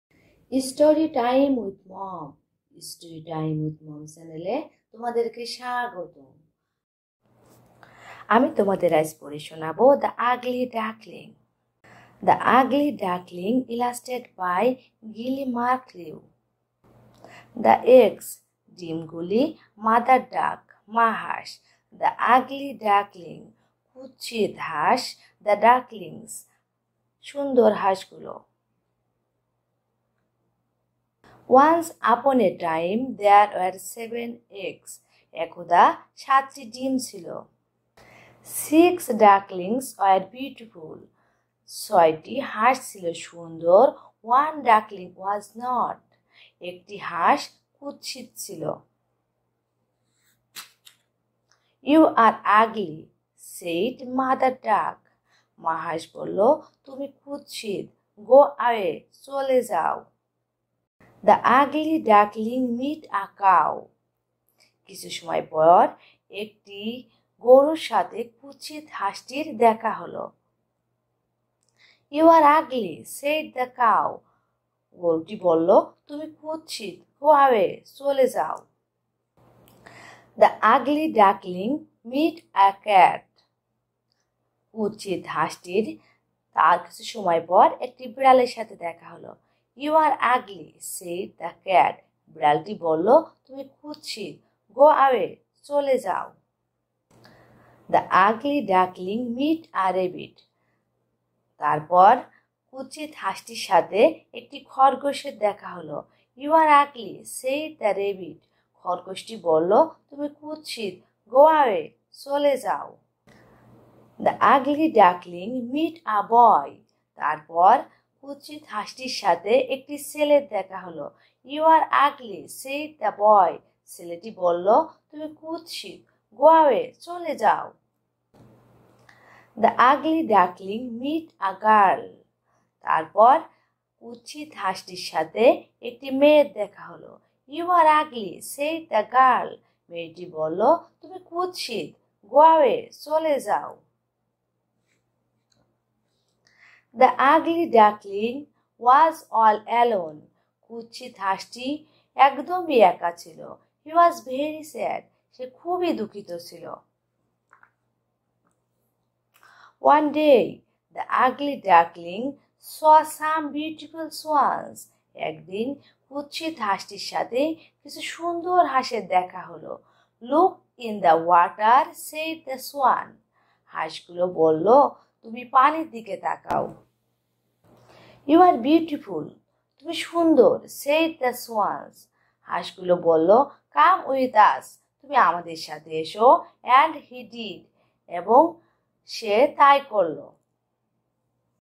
"The Ugly Duckling"- Read Aloud by Gilly Marklew
The Ugly Duckling Story.The Ugly Duckling. Long time ago, a farmer had a duck, which laid 7 eggs. After a few days all eggs hatched. Six ducklings looked liked their mom and the ...